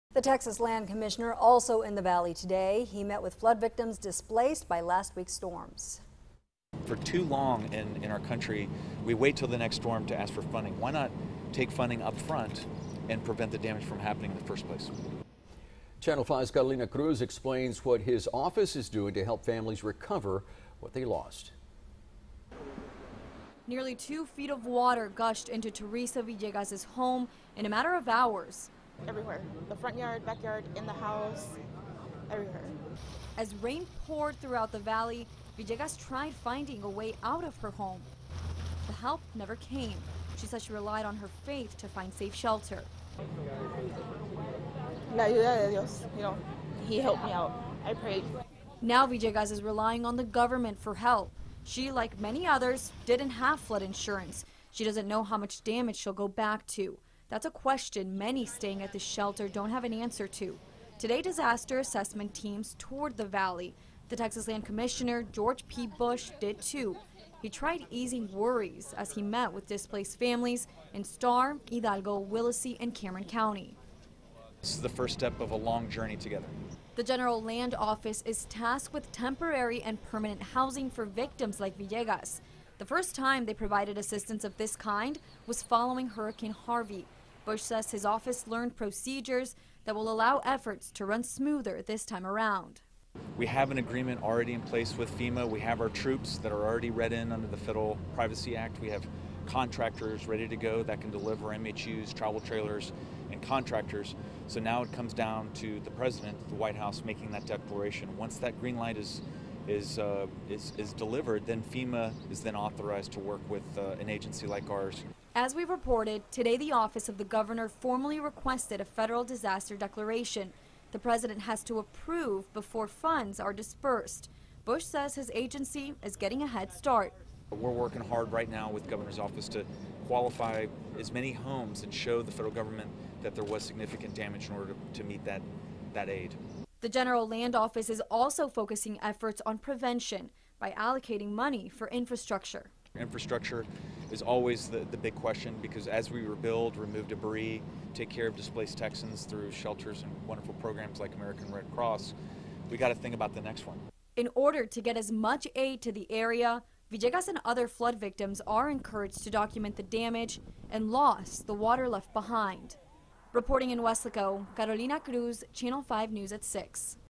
Texas Land Commissioner Surveys Flood Damage
He met with flood victims in Starr, Hidalgo, Willacy, and Cameron Counties who were displaced as a result of the floods.